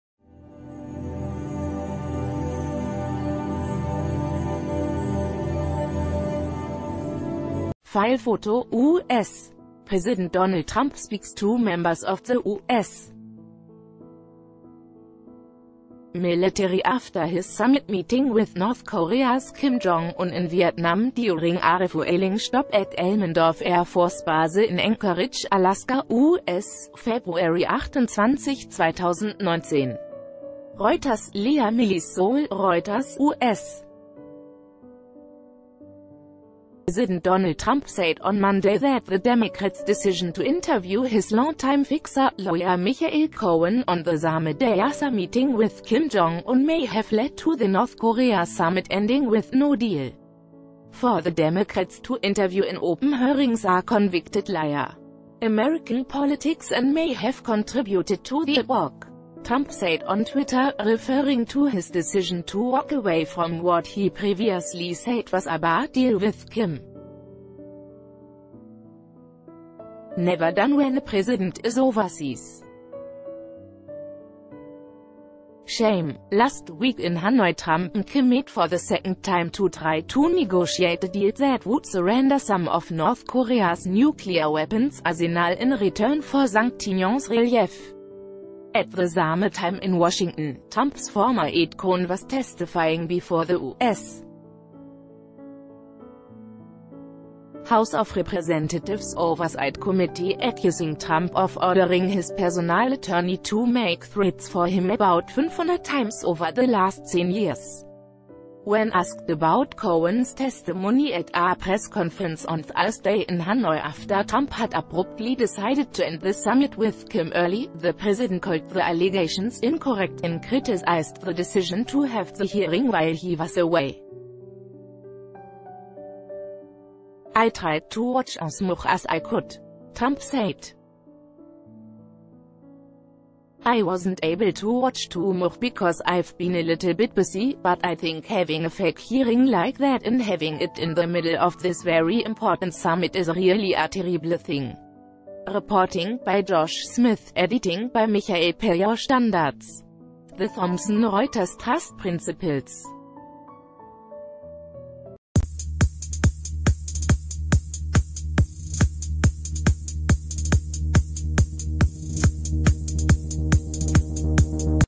Trump suggests Cohen hearing contributed to failure of North Korea...
FILE PHOTO: U.S. President Donald Trump speaks to members of the U.S. military after his summit meeting with North Korea's Kim Jong Un in Vietnam during a refueling stop at Elmendorf Air Force Base in Anchorage, Alaska, U.S., February 28, 2019. REUTERS/Leah MillisSEOUL (Reuters) - U.S. President Don...